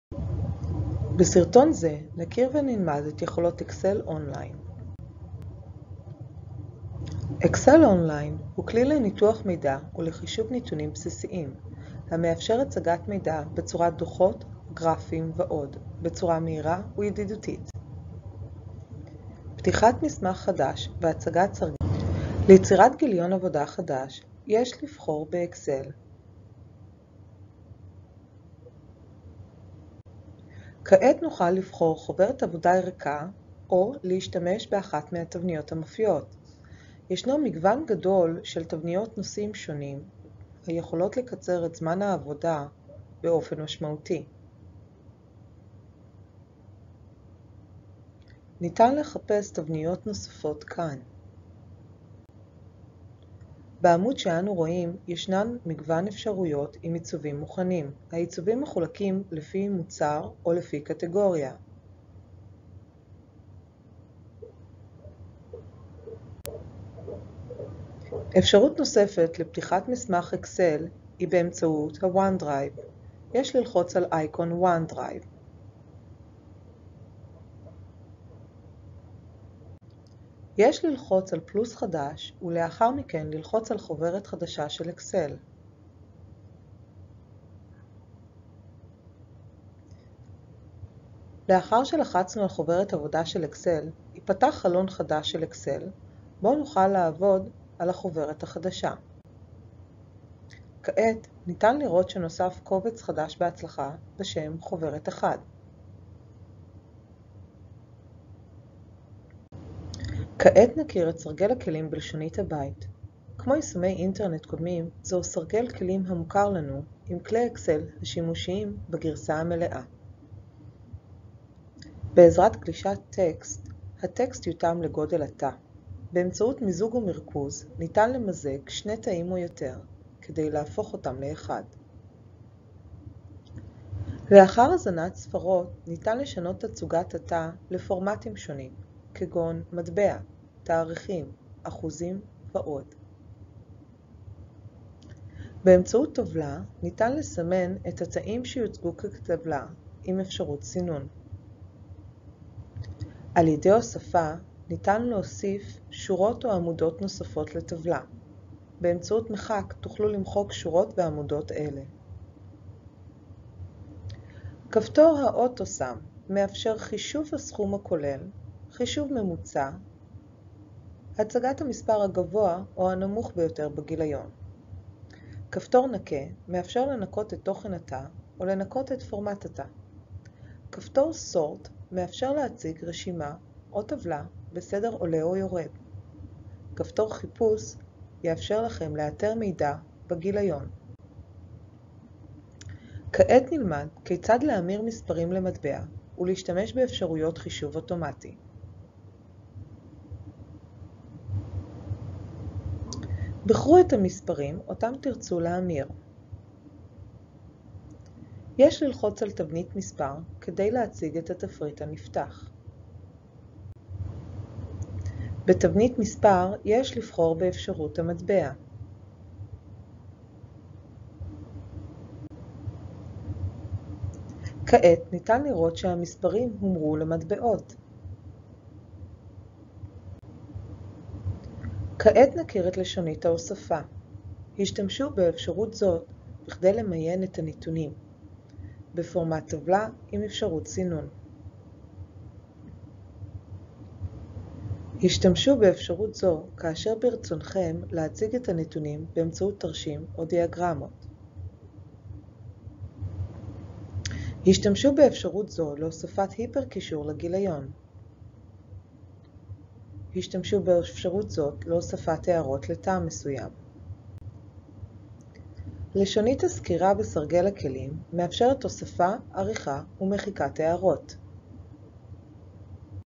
פתיחת מסמך חדש והצגת סרגל כלים Excel online
בסרטון זה נלמד  כיצד  ניתן לפתוח מסמך  ב-Excel online, ונסקור  את סרגל הכלים של היישום. סדרת סרטונים זו מיועדת לאנשי חינוך המעוניינים ללמוד על שימושי Office365 לחינוך.